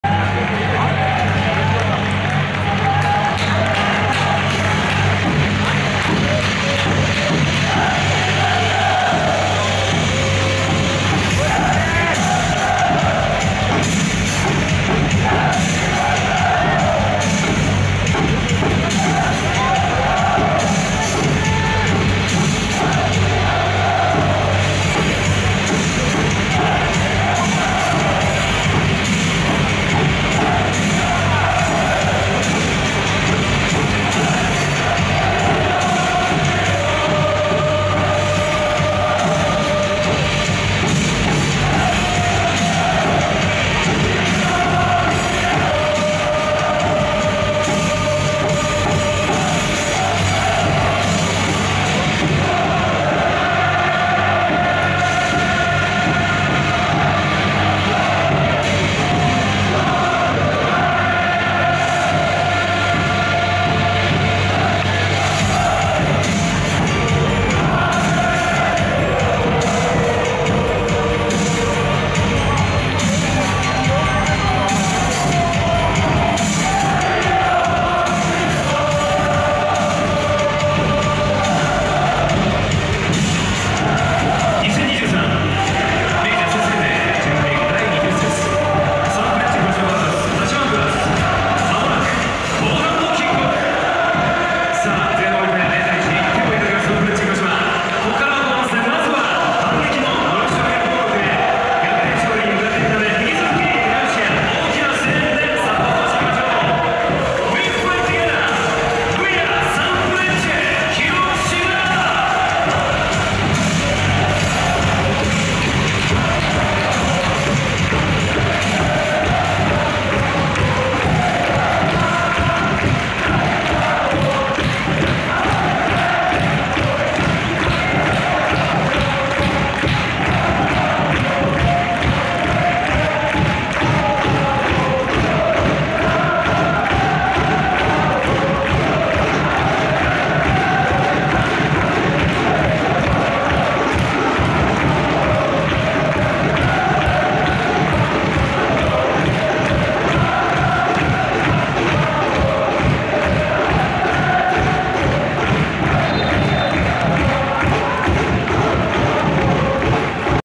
【ハーフタイム】後半キックオフ前の円陣⚽️⚽️ 2023.07.08 J1 第20節 鹿島アントラーズ 戦
キックオフ直後までは、モリシにプチ密着⚽

GK：大迫敬介
DF：中野就斗　松本大弥
MF：茶島雄介　野津田岳人　東俊希　柏好文　森島司
FW：ナッシムベンカリファ